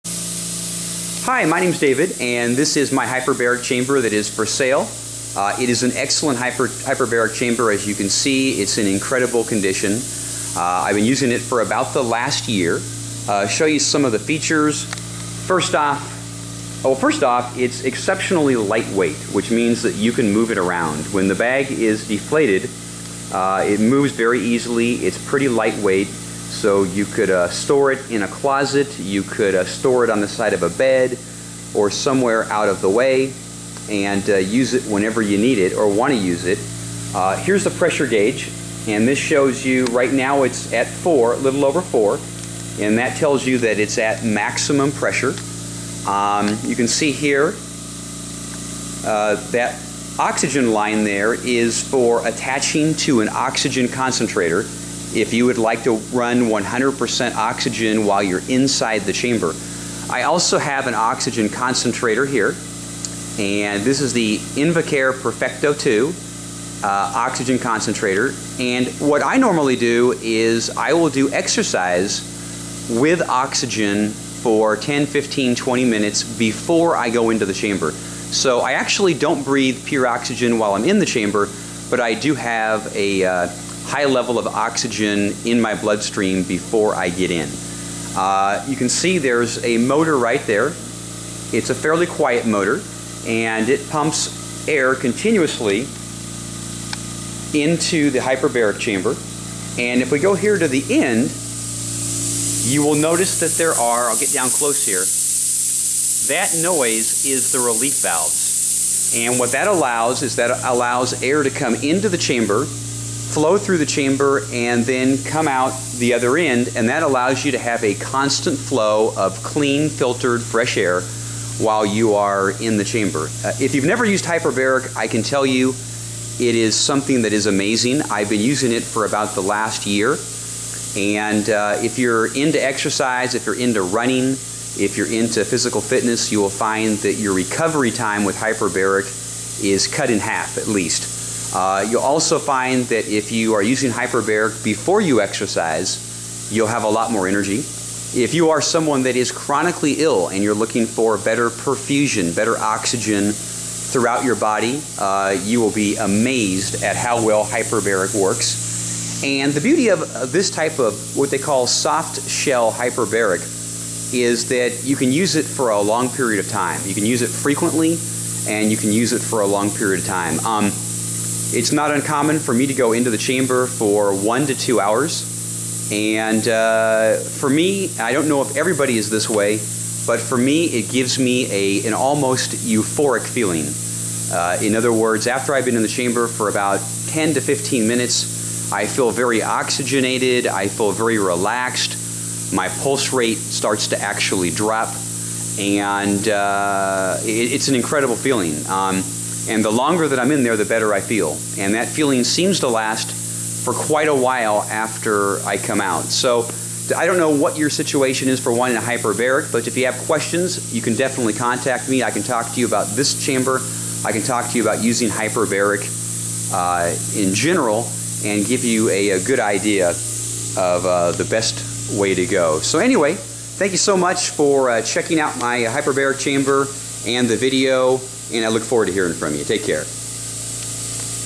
Hyperbaric Chamber For Sale Oxygen Concentrator
UPDATE 2025:  I sold this hyperbaric chamber many years ago. I used it to help with chronic illness and it did help a little. I found I had much better results with major diet changes and life changes. I now eat a keto / carnivore diet 80% to 90% of the time, and eat almost all organic. It has taken years, but this type of diet has helped me in many ways.

Oxygen Therapy With Exercise

I still use oxygen therapy with exercise and this seems to help me more than the hyperbaric ever did. This involves using an oxygen concentrator and breathing 80% to 90% oxygen while riding a stationary bike or some other exercise that gets your heart rate up. I use a face mask to breathe the oxygen and I use a 10 LPM oxygen concentrator. This setup is cheaper than buying a hyperbaric chamber and seems to make me feel better. If you are too ill or weak to do this, you may be better off with just using a hyperbaric chamber by itself.

Hyperbaric Chamber and Oxygen Concentrator Questions and Answers

1. What is a soft-side hyperbaric chamber, and how does it work?

My hyperbaric chamber was soft and flexible and could actually be folded up and put in a box. It creates an environment with increased pressure that allows your body to absorb more oxygen than it would at normal atmospheric pressure. This helps accelerate the body’s natural healing processes and improve overall health. I will say I always felt better after a session. I would breathe oxygen from my oxygen concentrator while inside the chamber. I did this by running a small tube through a hole in the chamber that was designed for this purpose.

2. How does the oxygen concentrator enhance the therapy?

The oxygen concentrator filters and concentrates the oxygen in the air, delivering a higher oxygen level to the chamber. My concentrator was a 10 LPM and I was told it would deliver about 80% to 90% oxygen. It is not pure oxygen but more concentrated than room air. By increasing the oxygen levels inside the chamber, it maximizes the therapeutic benefits of hyperbaric oxygen therapy, helping your body heal faster and more effectively.

3. Is using a soft-side hyperbaric chamber safe?

I used my hyperbaric chamber multiple times each week and stayed inside for about an hour each time. I remember reading about NFL players who would sleep all night in their chamber the night before a game.  You can do this with a soft-sided hyperbaric chamber because the PSI is lower than an HBOT used by a professional company.

4. How long should a session in the hyperbaric chamber last?

Like I said, I used my hyperbaric chamber frequently, and sometimes 7 days a week at times. I would average about an hour, but also did many shorter and longer sessions. The longest I ever stayed in was I think 2 hours. I could have stayed longer but it gets boring in there and I was hungry :-). I would start with shorter sessions and build up when you feel ready.

5. Does Oxygen and Hyperbaric Give You More Energy?

I definitely noticed more energy. I also notice a boost to energy when I do exercise with oxygen therapy. I'm sure like everything else, your mileage may vary. I have met people who think HBOT is the greatest thing they have ever done, and others who felt no difference at all.

You should probably speak with a Doctor and tell them what you are doing before you start HBOT.

If you have other questions, put them in the comments section and I will do my best to answer them. I am not logged into this account all the time.